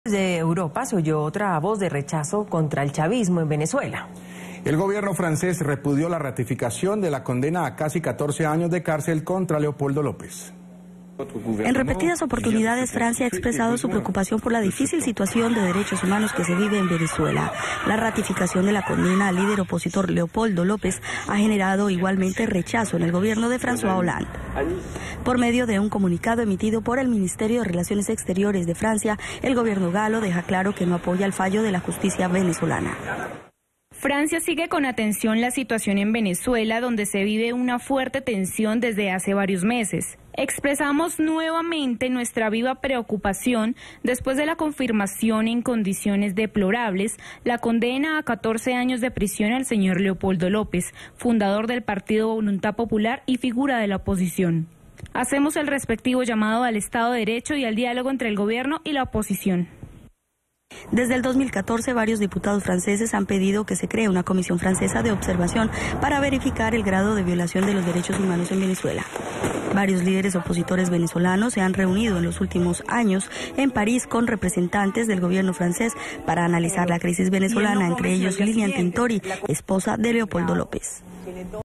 "Instamos al respeto del Estado": Gobierno francés preocupado por condena a Leopoldo López
El Ministerio francés de Exteriores dijo seguir "con atención" la situación en Venezuela al expresar su preocupación tras la confirmación de la condena a casi 14 años de prisión contra el opositor Leopoldo López.